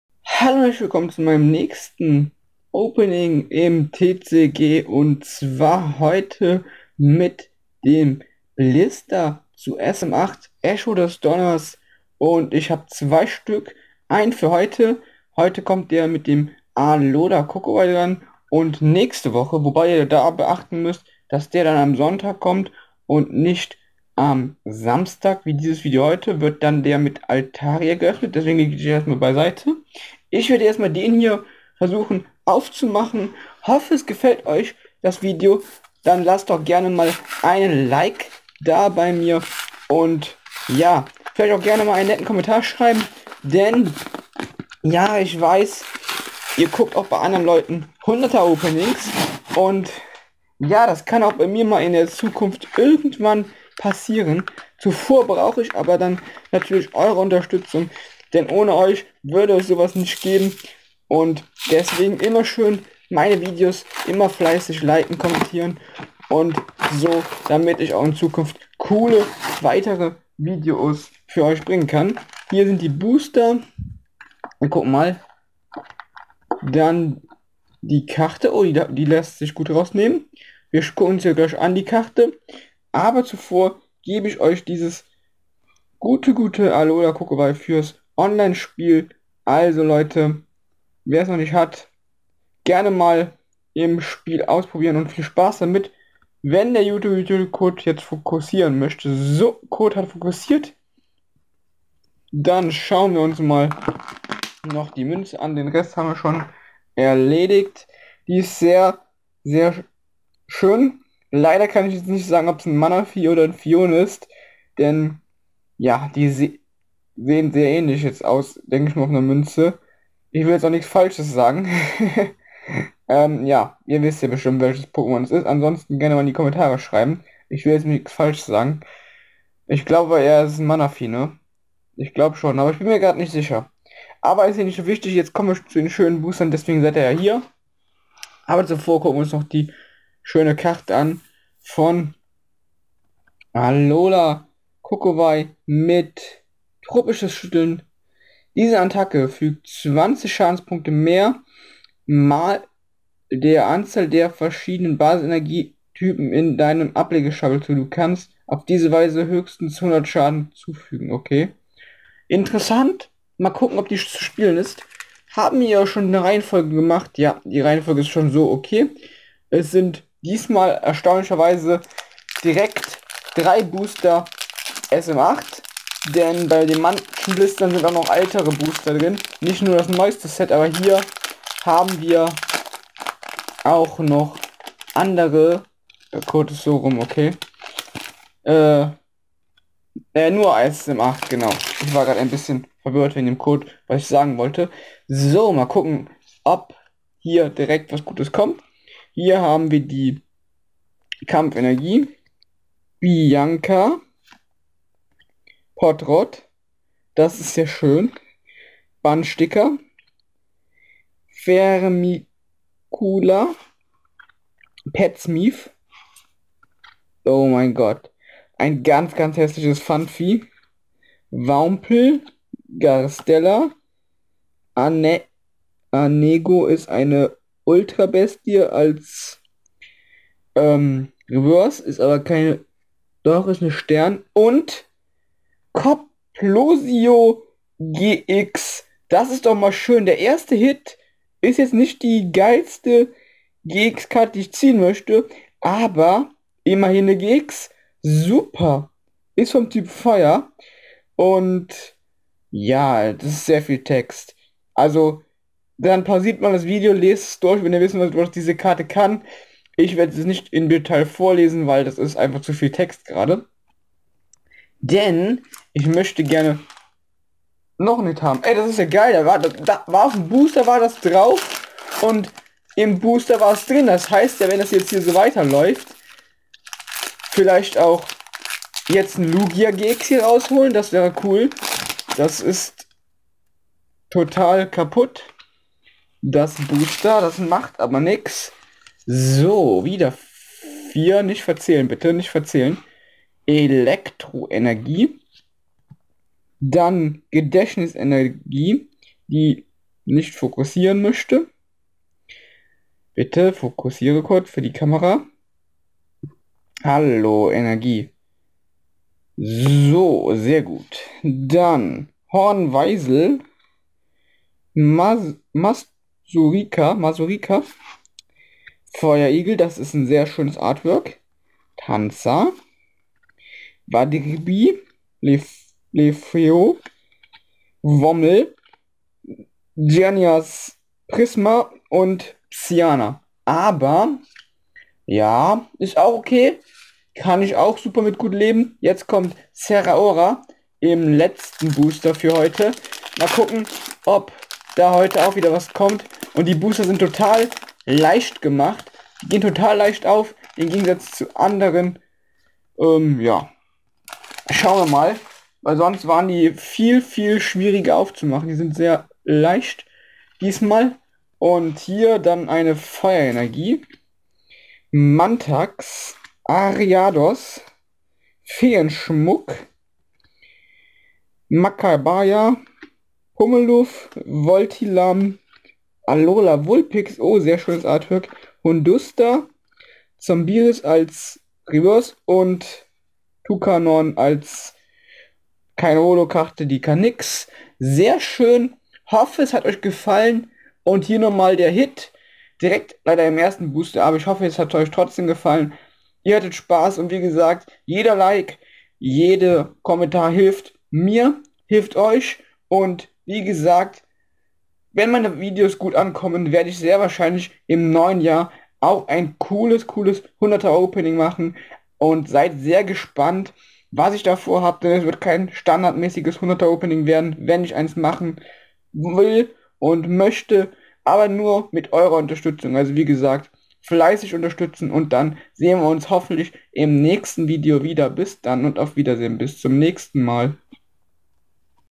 Pokemon TCG Opening Alola Kokowei 3-Pack Blister SM8 Echo des Donners
Pokemon TCG
Heute gibt es ein 3-Pack Blister mit der Promo-Karte von Alola Kokowei